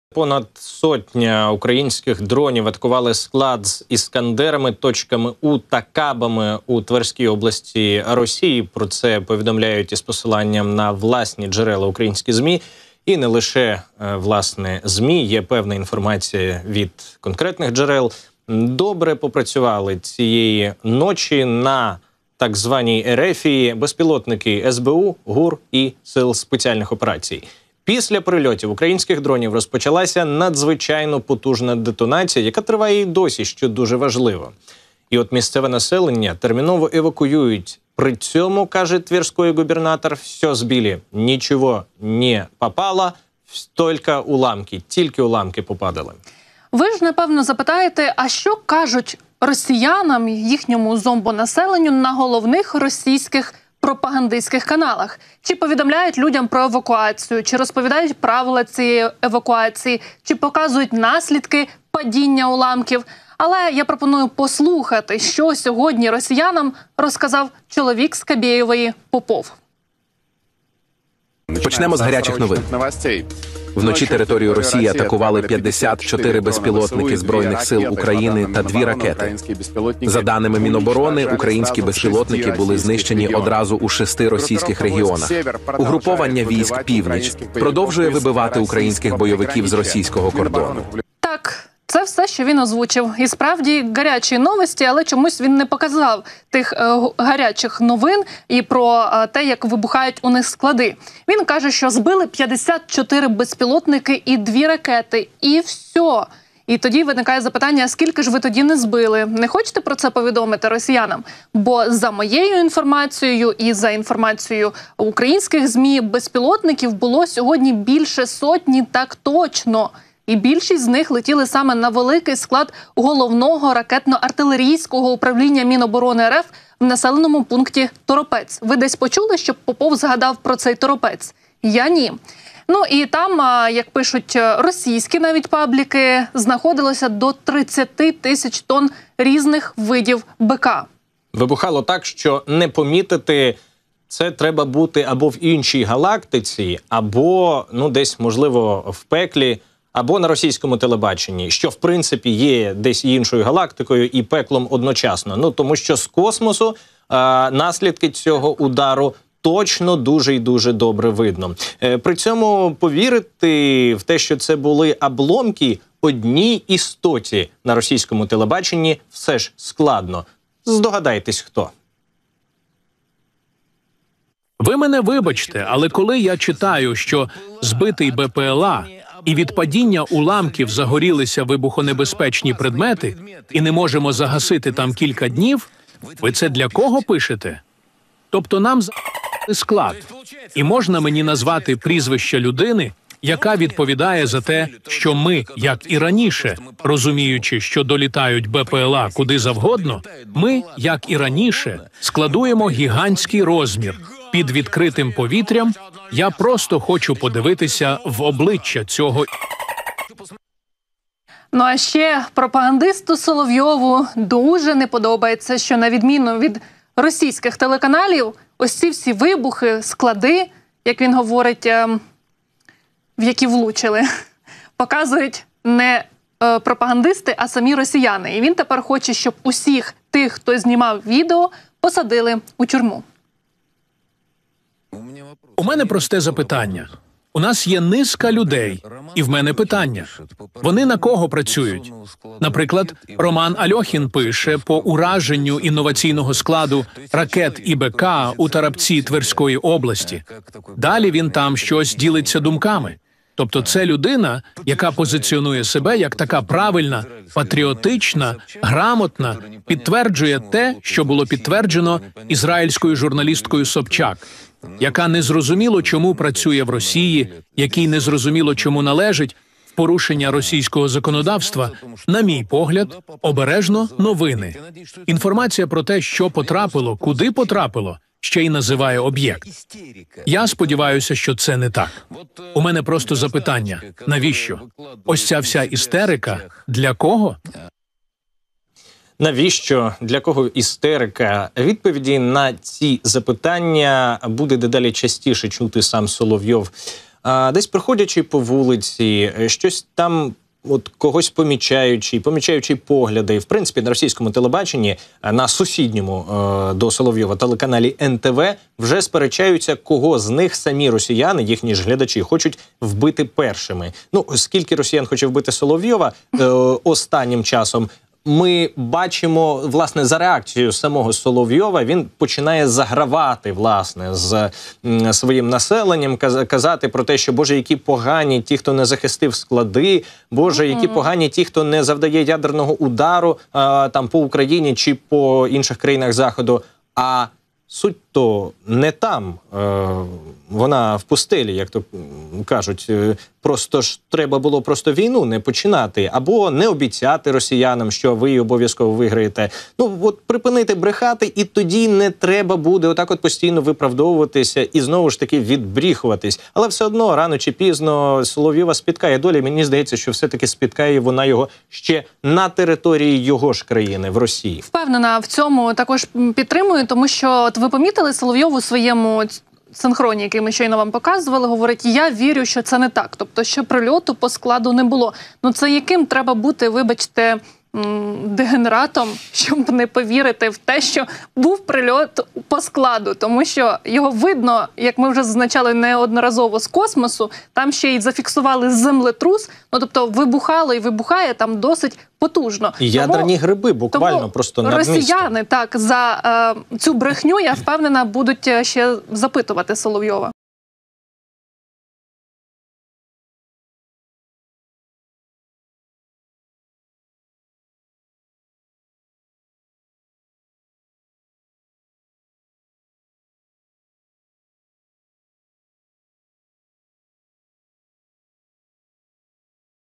Лише ПОГЛЯНЬТЕ: у Соловйова ПРИПАДОК через ПРИЛЬОТИ по Торопцю. Пропагандист мало НЕ ЗАПЛАКАВ
Уночі 18 вересня у місті Торопець Тверської області пролунали потужні вибухи. За попередньою інформацією, дрони уразили склад з боєприпасами, спалахнули масштабні пожежі. Вибухова хвиля від детонації була такої потужності, що споруди у навколишніх населених пунктах залишилися без вікон та дверей. Російська влада оголосила евакуацію. Тим часом російське телебачення не отримавши методички з Кремля почало всіляко замовчувати факт удару українських БПЛА. Не змовчав лише Соловйов, влаштувавши справжню істерику в ефірі своєї передачі. Детальніше про це – дивіться у відео.